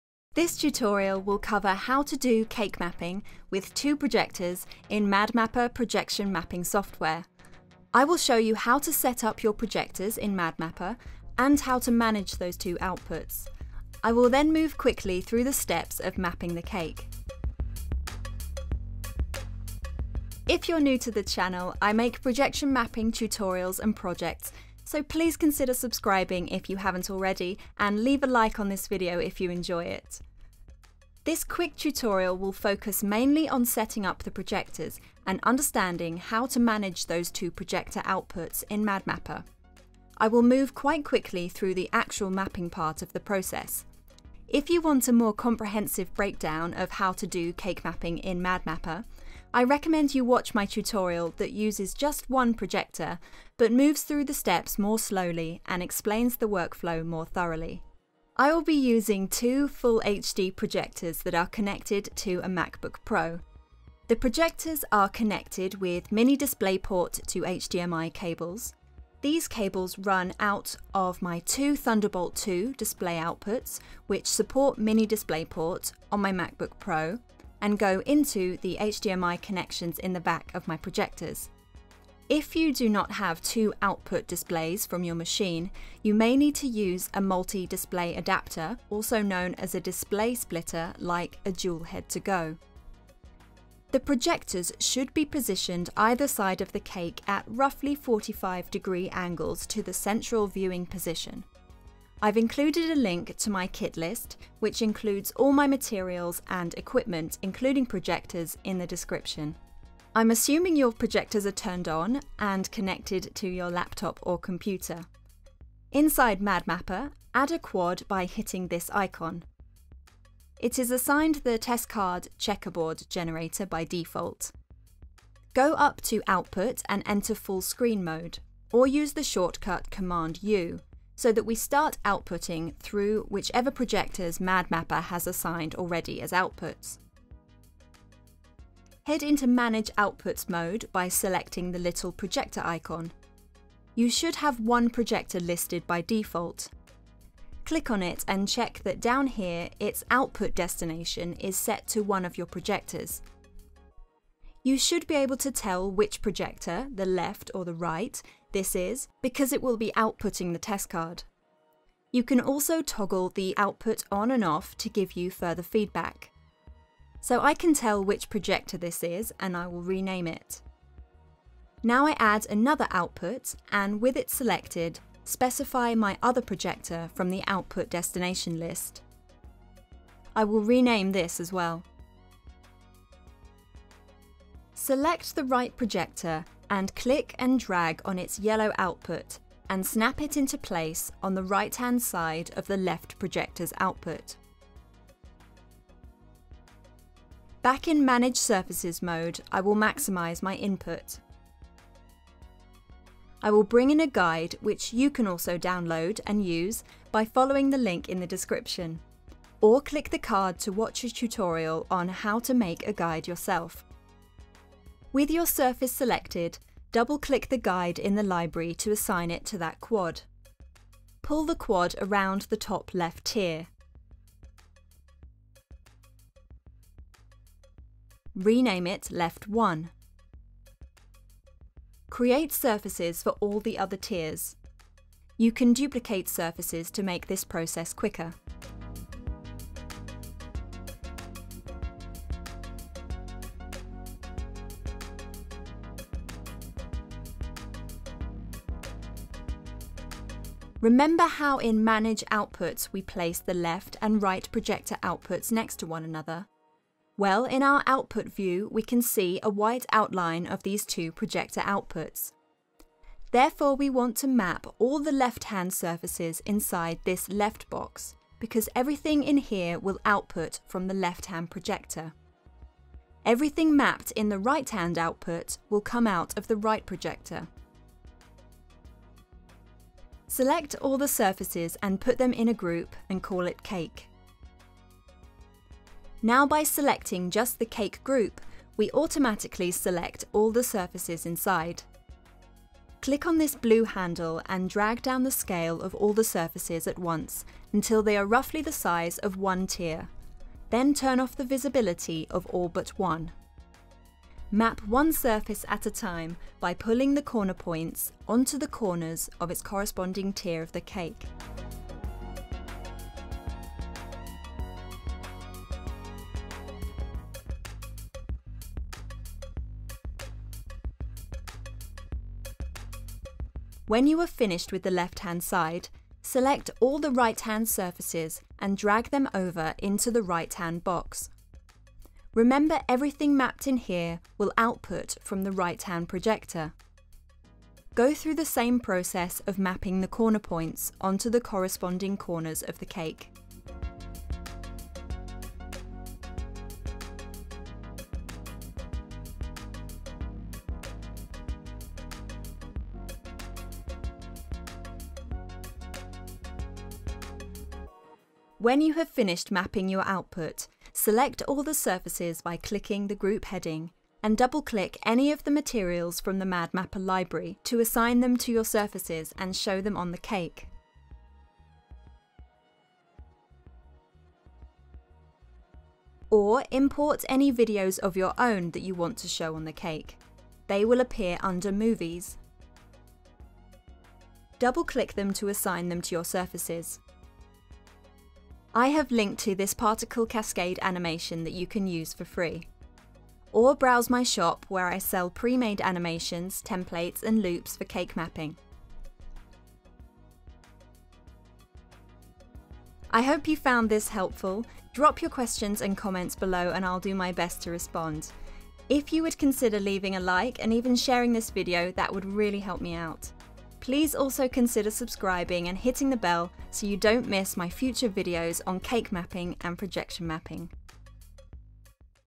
Cake Mapping with Multiple Projectors MadMapper Tutorial
Learn how to do cake mapping with multiple projectors in MadMapper projection mapping software. I will show you how to set-up two projectors in MadMapper. I will then go through the steps of video mapping the cake.

Timecodes:
00:00 Introduction
00:58 Hardware & Equipment
01:57 Setting Up Two Projectors
03:21 Cake Mapping in MadMapper